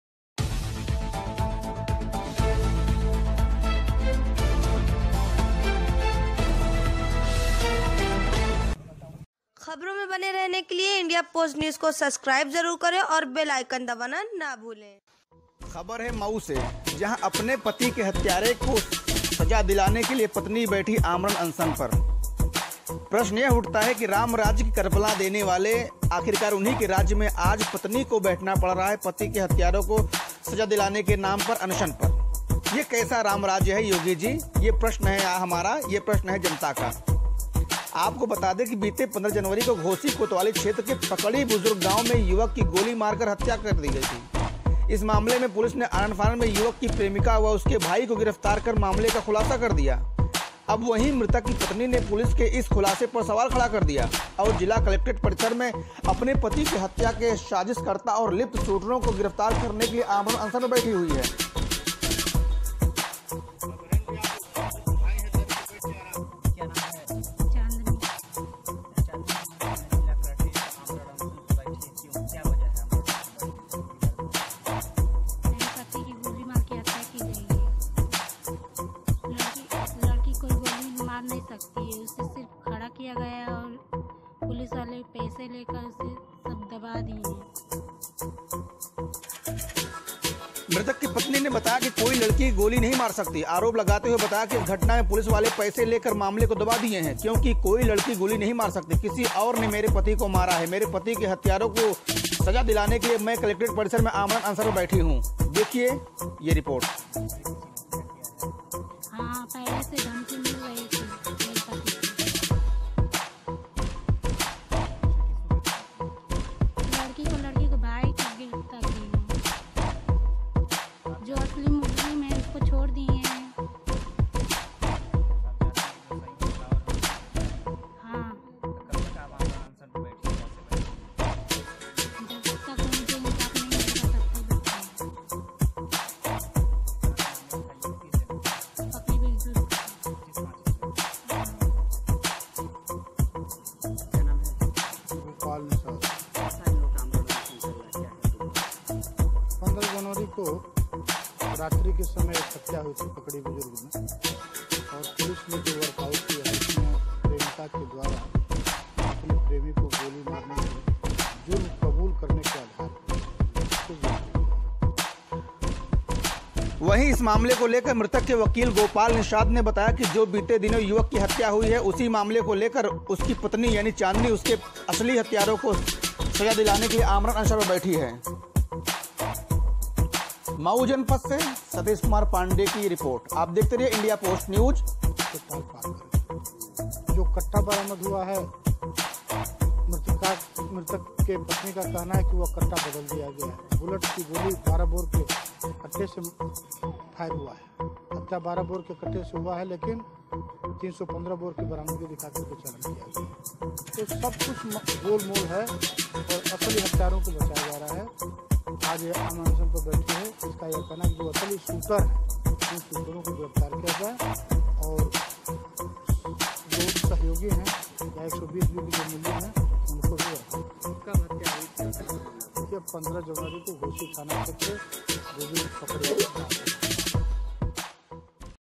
अपने पति के हत्यारे को सजा दिलाने के लिए पत्नी बैठी आमरण अनशन पर
सतीश कुमार पांडे मऊ

स्लग- अपने पति के हत्यारे को सजा दिलाने के लिए पत्नी बैठी आमरण अनशन ।

एंकर-  बीते 15 जनवरी  को  घोसी कोतवाली क्षेत्र के पकड़ी बुजुर्ग गांव में युवक की गोली मारकर हत्या का मामला सामने आया था। इस मामले में पुलिस ने आनन फानन में युवक की प्रेमिका व उसके भाई को गिरफ्तार कर मामले का खुलासा कर दिया । अब वही मृतक की पत्नी ने पुलिस के इस खुलासा पर सवाल खड़ा कर दिया है। और जिला कलेक्ट्रेट परिसर में अपने पति के हत्या के साजिशकर्ता और लिप्त शूटरों को  गिरफ्तार करने के लिए  आमरण अनशन पर बैठ गई है ।

वी0 ओ0 - मृतक की पत्नी ने बताया कि कोई  लड़की गोली नहीं मार सकती आरोप लगाते हुए बताया कि  इस घटना में पुलिस वाले पैसे लेकर मामले को दबा दिए हैं। क्योंकि कोई लड़की गोली नहीं मार सकती किसी और ने  मेरे पति को मारा है मेरे पति के हत्यारों को सजा दिलाने के लिए मैं कलेक्ट्रेट परिसर में आमरण अनशन पर बैठी हुई हूं।

वीओ 2- वही इस मामले को लेकर मृतक के वकील गोपाल निषाद ने बताया कि जो बीते दिनों युवक की हत्या हुई है उसी मामले को लेकर उसकी पत्नी (चाँदनी) उसके असली हत्यारों को सजा दिलाने के लिए  आमरण अनशन पर बैठी हुई है।

बाइट- चांदनी, (मृतक की पत्नी)

बाइट- गोपाल निषाद, (वकील )